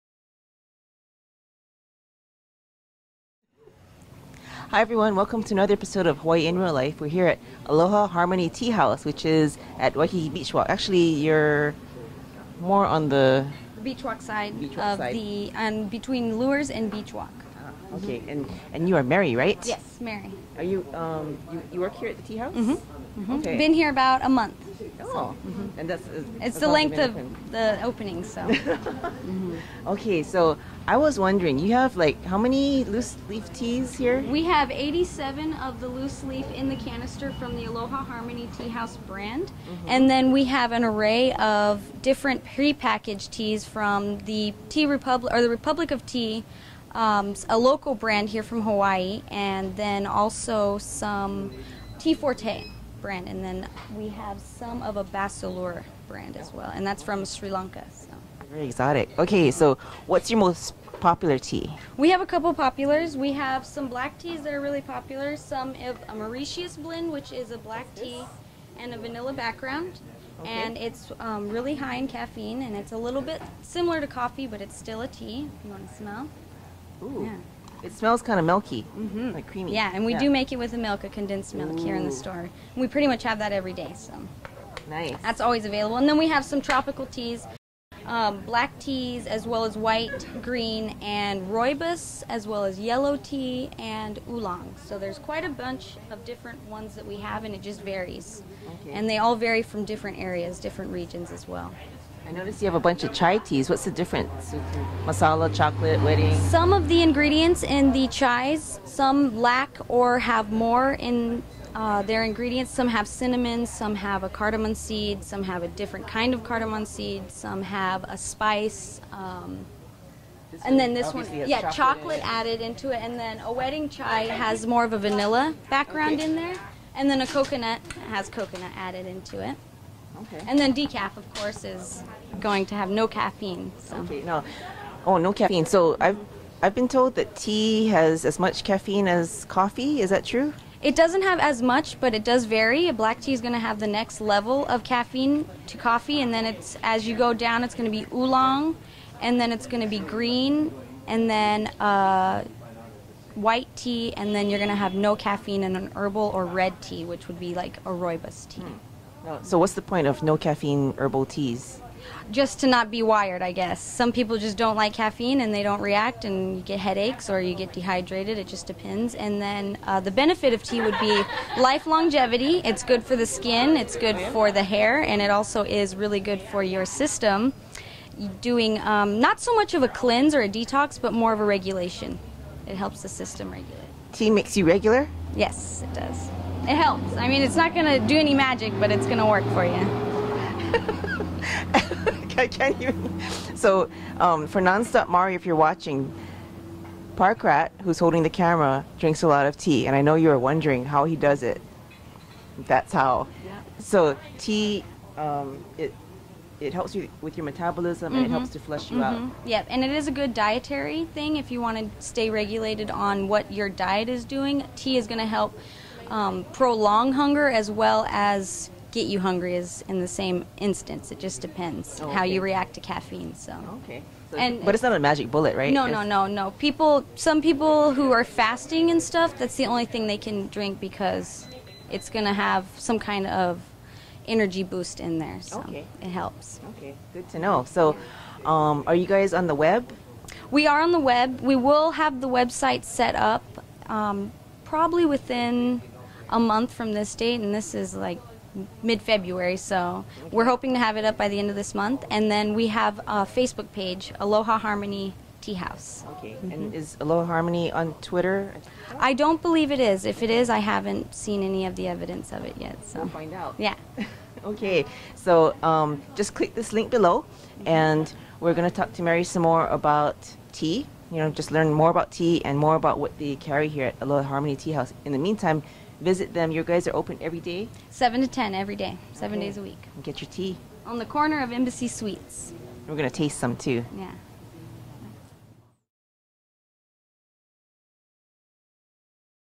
Aloha Harmony Tea House - Nonstop Segment
Melissa visits with Mary of Aloha Harmony Tea House.